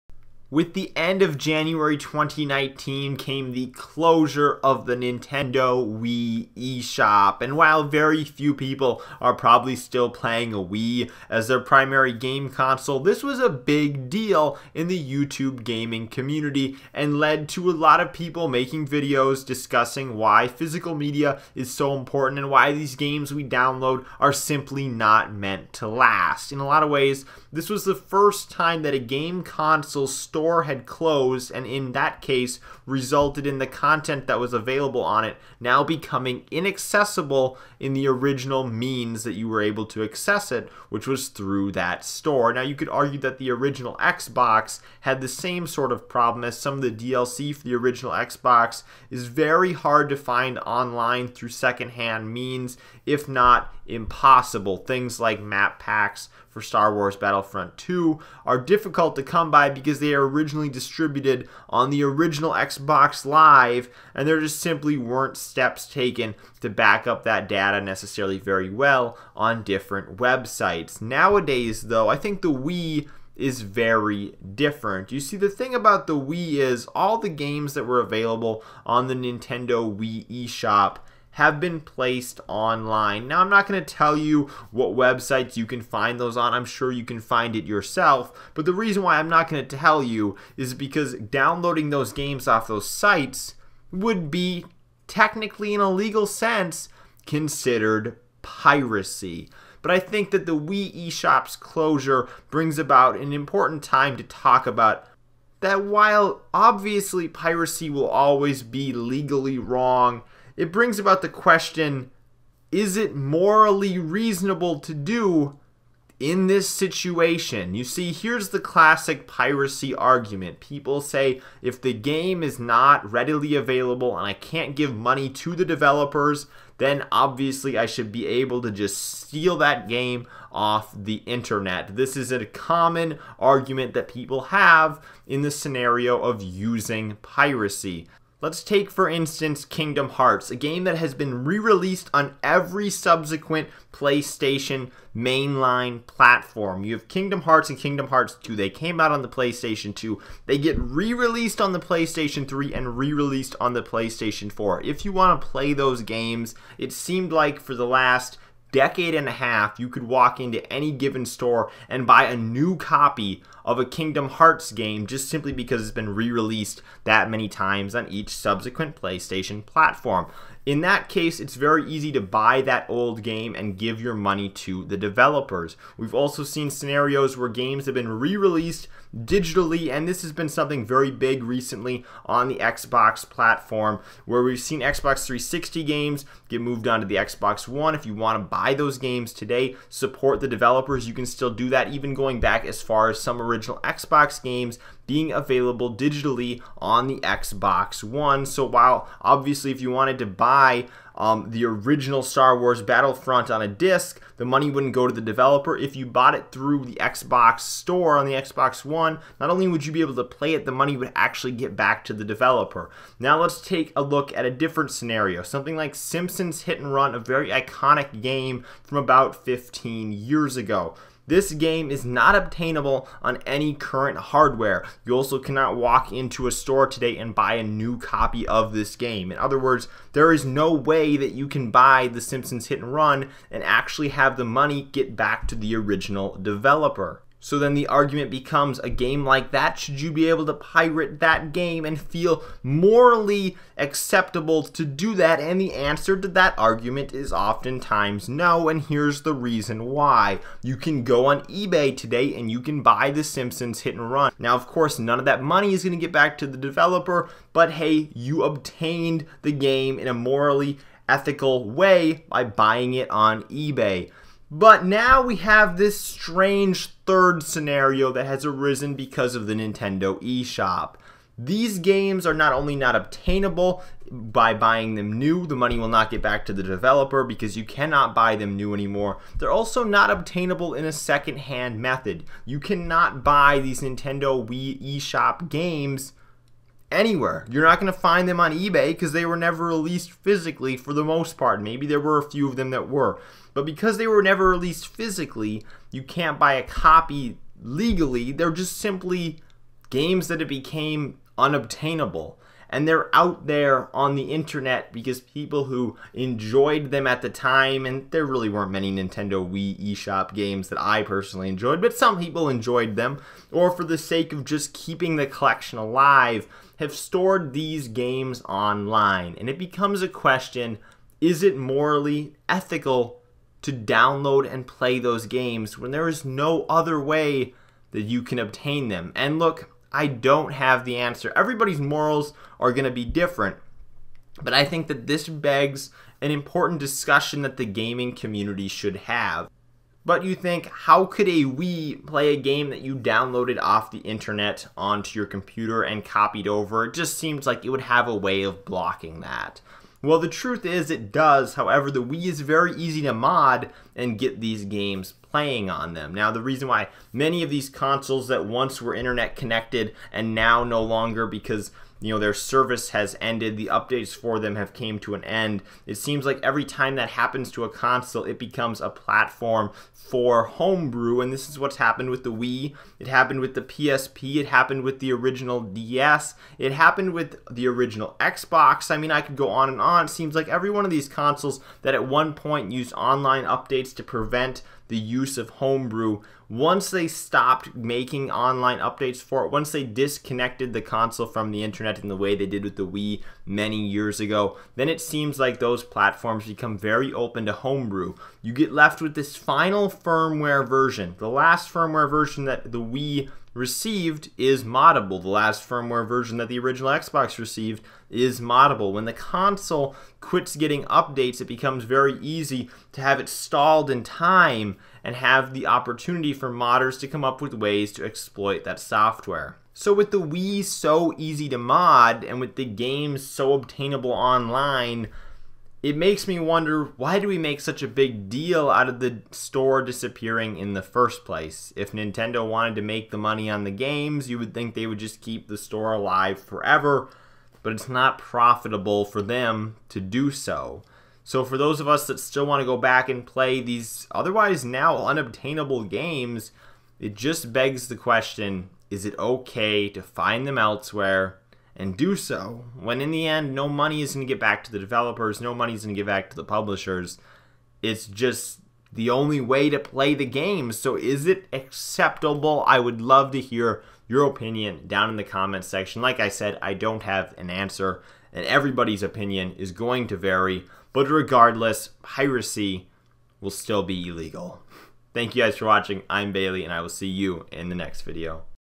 What the Wii Shop Closing Means for Piracy
The Wii Shop closed leaving many games released on the platform now simply un-buyable. There is no way to download these Wii Shop games in the way they were originally intended to be download in. However, this does not mean you can not download and play these Wii Shop games in other ways even in 2019 and beyond.

The Wii is a very easy console to mod and with it comes the ability to download games off a variety of website and play them on your console even without purchasing them. This is a form of piracy and obviously illegal. However, it remains the only way to play many of these games if you did not buy them during the Wii's run. Is this right, or is this the wrong thing to do? What can we do to prevent this same situation from coming up when another console (like the X360 or PS3) come to end and shut down their stores?

SNAPCHAT: im.bailey (Notice the dot after the m)
Xbox GT: Im SpawnTrapped